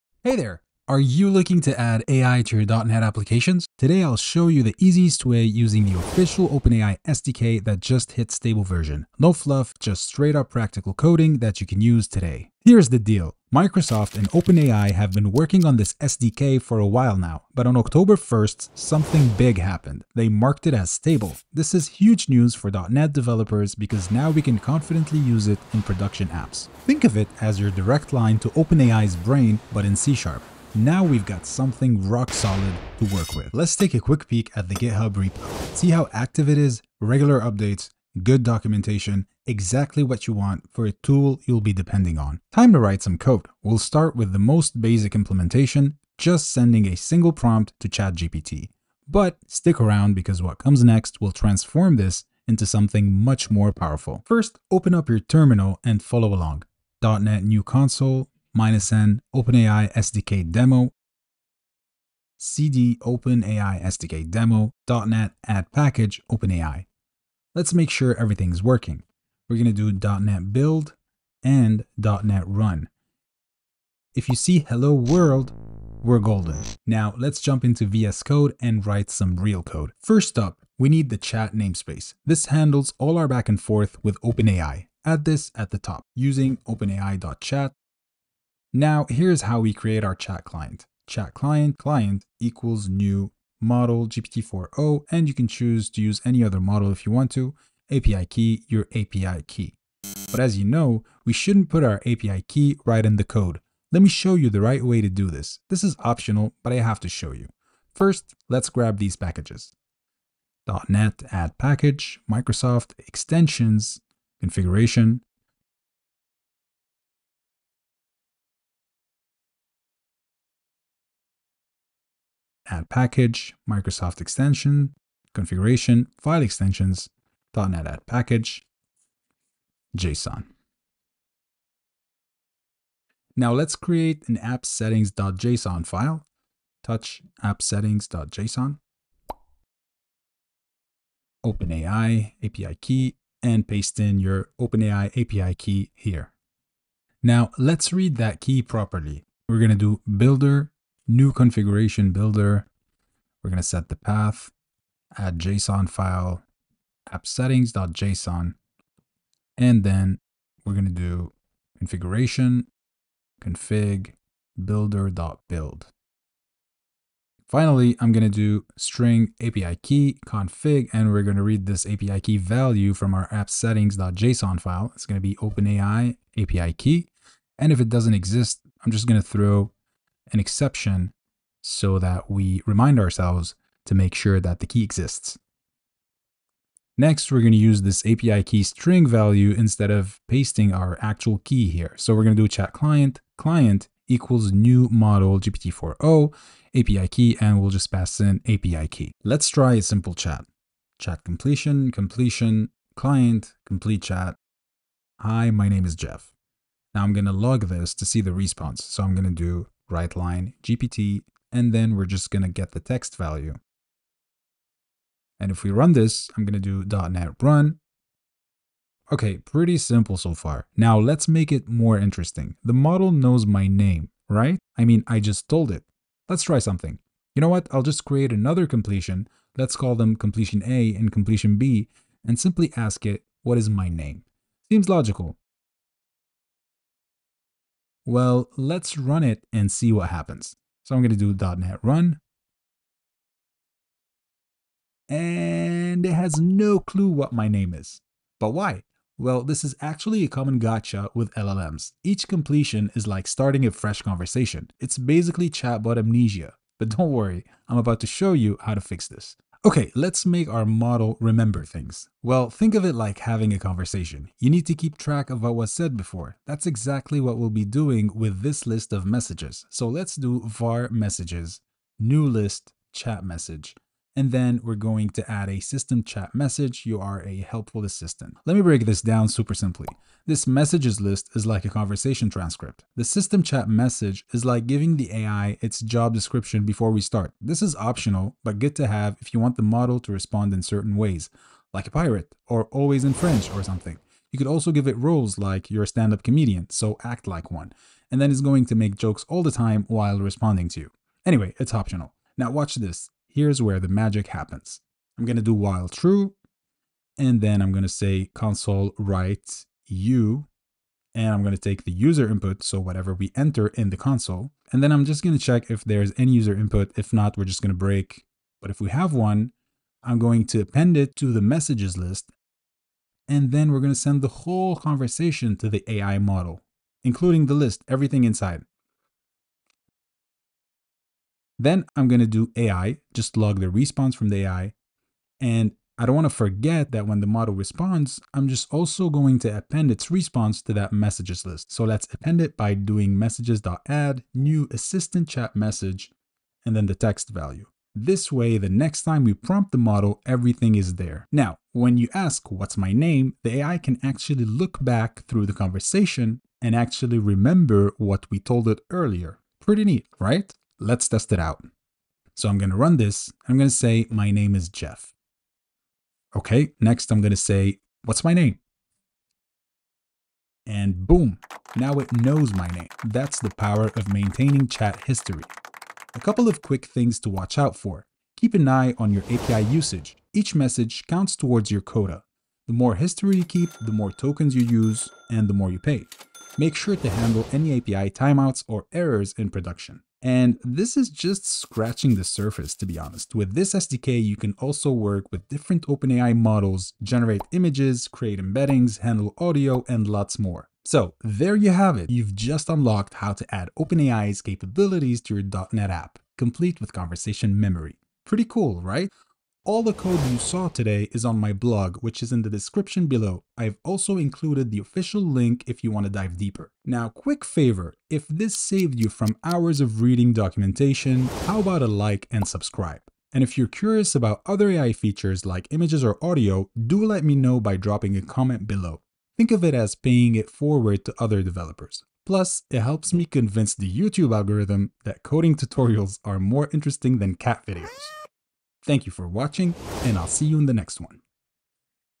OpenAI .NET SDK - Add ChatGPT to Your App in MINUTES!
Want to add ChatGPT capabilities to your .NET projects? Let's walk through building a real chat application using the official OpenAI .NET SDK. This guide works for both .NET Framework and .NET Core, whether you're building a console app, Web API, or something else entirely.

I'll show you how to:

- Get up and running with the SDK
- Create a basic but functional chat interface
- Maintain conversation context between messages
- Keep your API keys safe (and out of source control!)
- Handle common gotchas and edge cases

This is a hands-on tutorial - we'll write actual code and build something useful. No previous AI experience needed, just basic C# knowledge.

The Getting Started with AI Blog